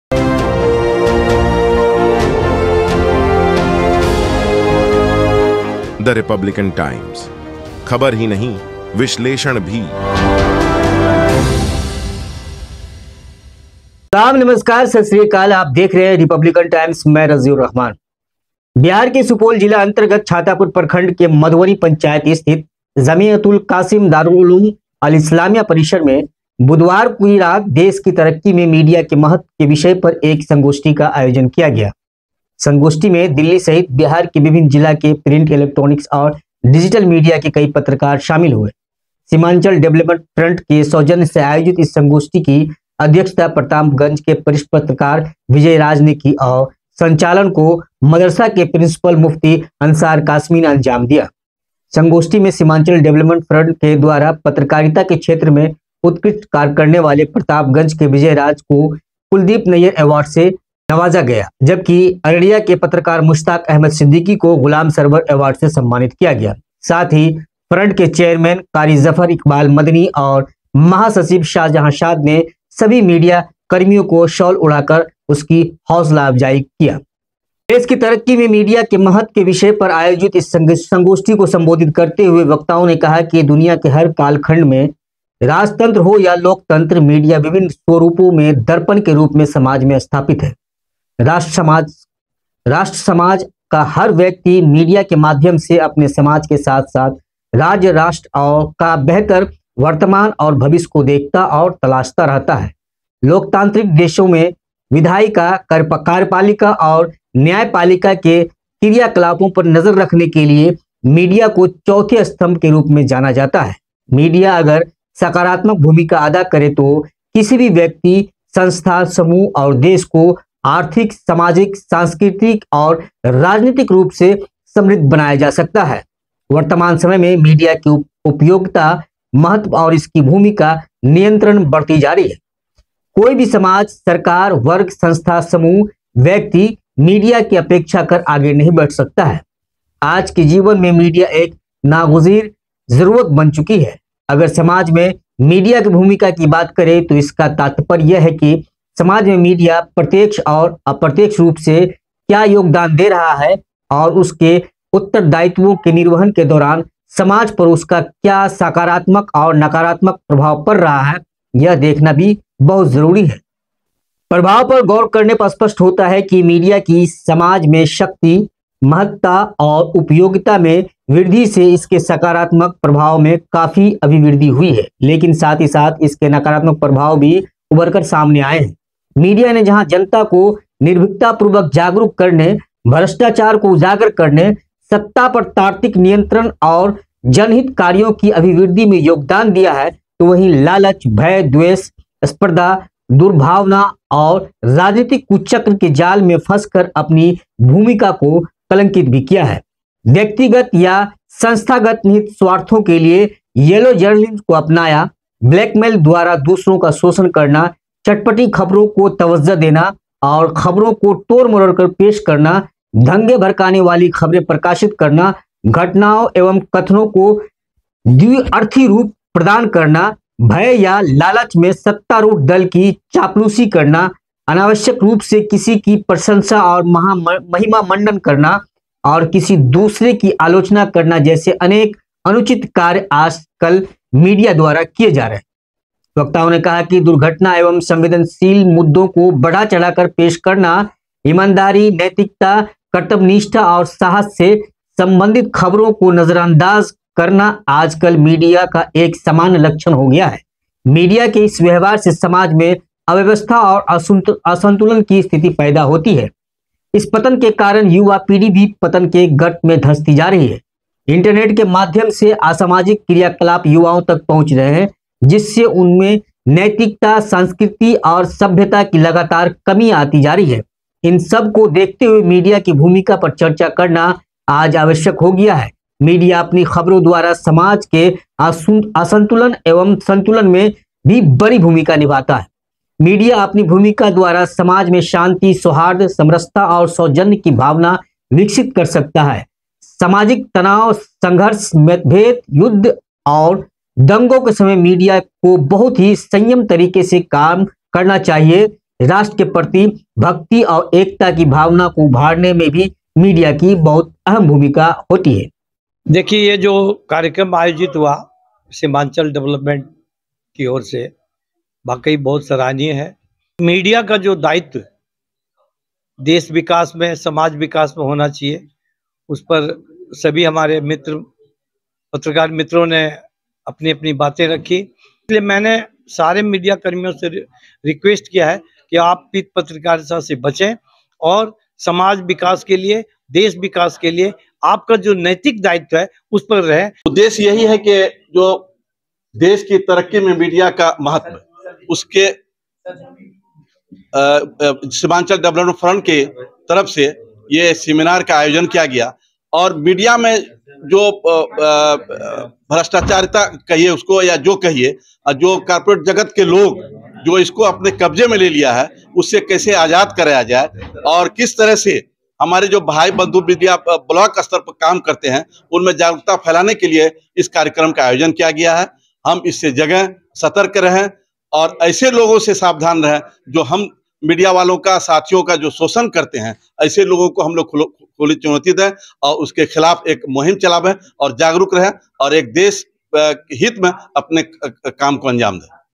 जमीयतुल कासिम दारुल उलूम-इल-इस्लामिया : पत्रकार संगोष्ठी सम्मानित हुए दिल्ली और बिहार के...
जमीयतुल कासिम दारुल उलूम-इल-इस्लामिया : पत्रकार संगोष्ठी सम्मानित हुए दिल्ली और बिहार के दर्जनों पत्रकार
Hi,
Welcome to The Republican Times YouTube News channel
About US :
Digital Hindi Daily Newspaper  & YouTube News Channel I
Thank you so much.